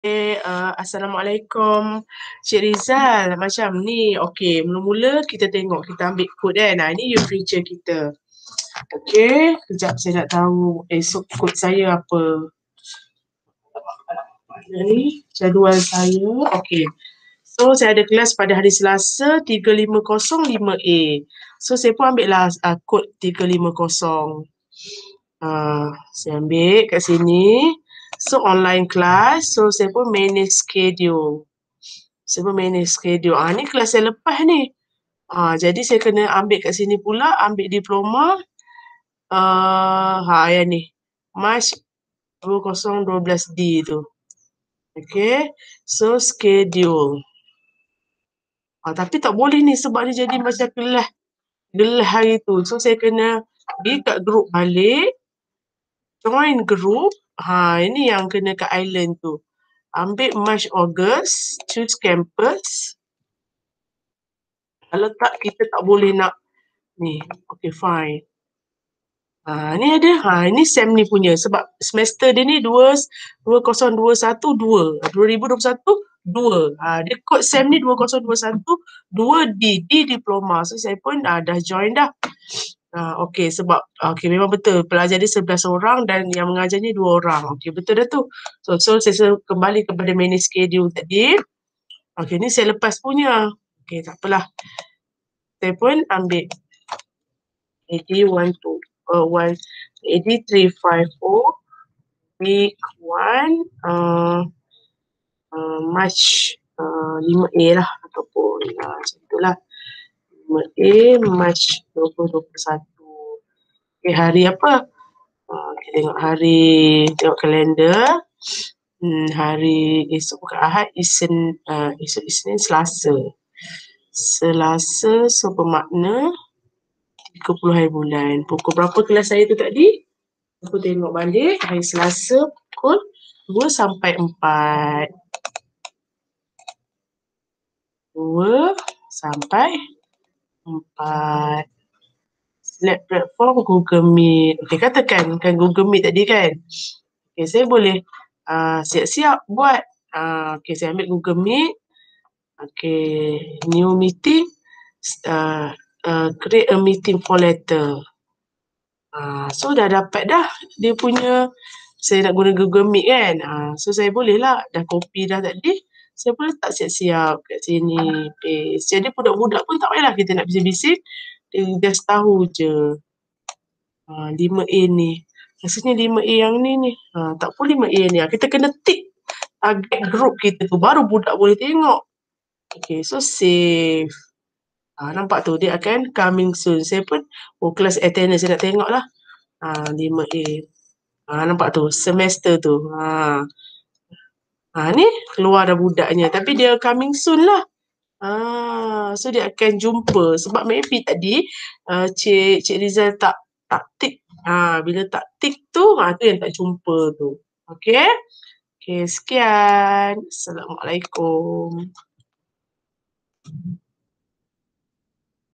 Cara Create Attendance di UFuture
Mudah create attendance di Ufuture sebab nama pelajar di Ufuture dijana dari SIMS UiTM.